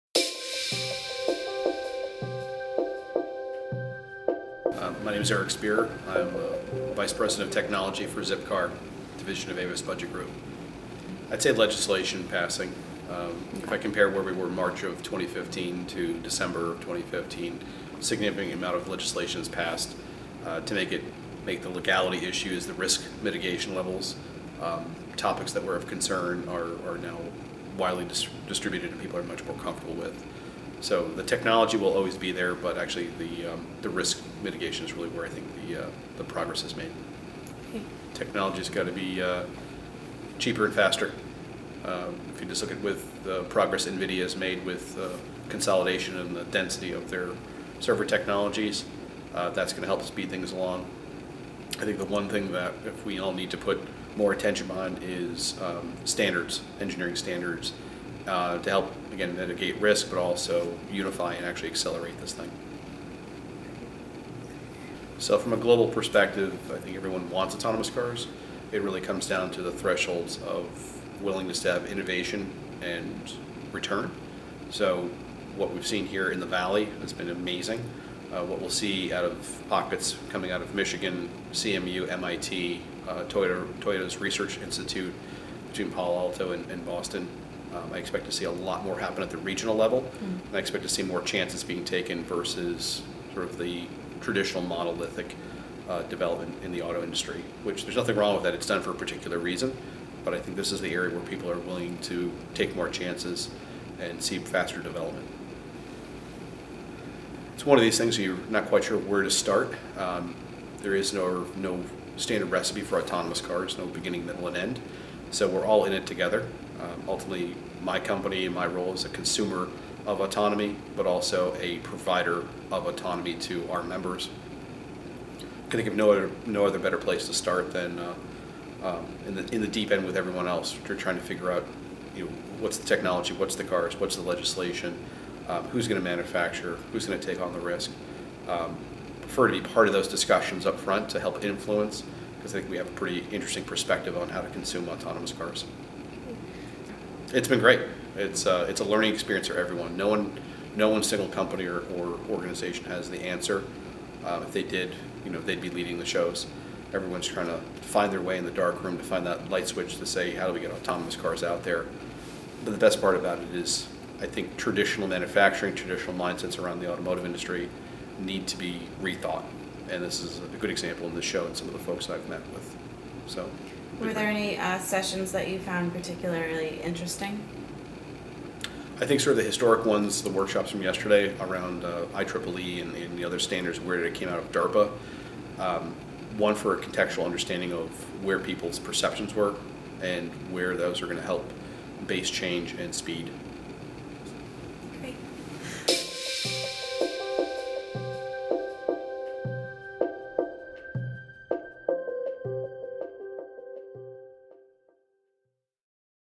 The Changing Landscape of Autonomous with Zipcar
Eric Spear, Vice President of Technology at Zipcar, speaks about legislation and standards in the autonomous industry, key highlights/major accomplishments of 2015 for the industry, top predictions for the autonomous vehicle in the next 5 years, progression on a regional vs. global level, and the value of attending Autonomous Cars 2016.